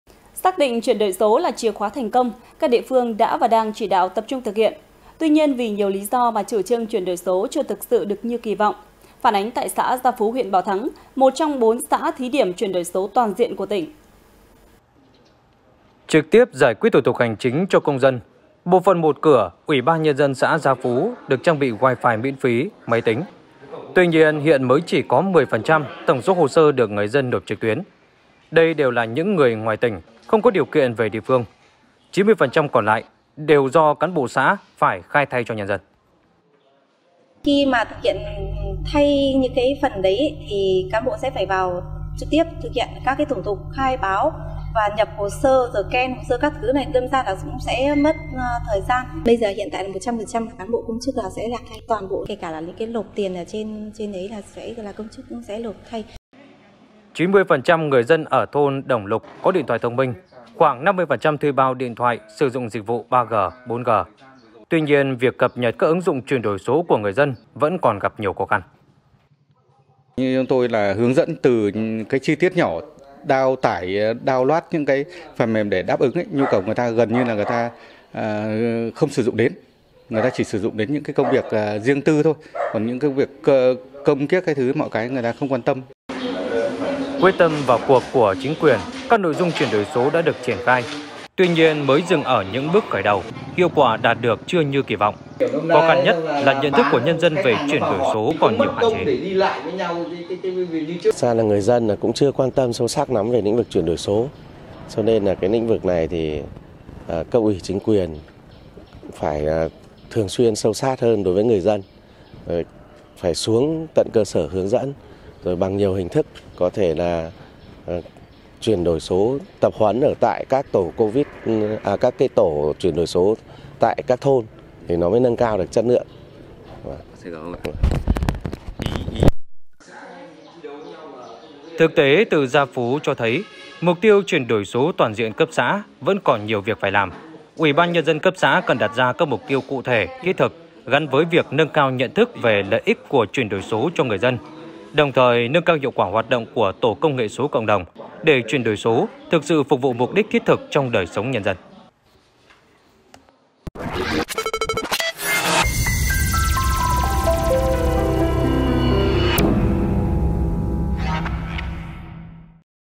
Chuyển đổi số toàn diện cấp xã còn khó khăn| THLC
Laocaitv.vn - Xác định chuyển đổi số là chìa khóa thành công, các địa phương đã và đang chỉ đạo tập trung thực hiện. Tuy nhiên, vì nhiều lý do mà chủ trương chuyển đổi số chưa thực sự được như kỳ vọng.  Phản ánh tại xã Gia Phú, huyện Bảo Thắng, 1 trong 4 xã thí điểm chuyển đổi số toàn diện của tỉnh.

Đài Phát thanh - Truyền hình Lào Cai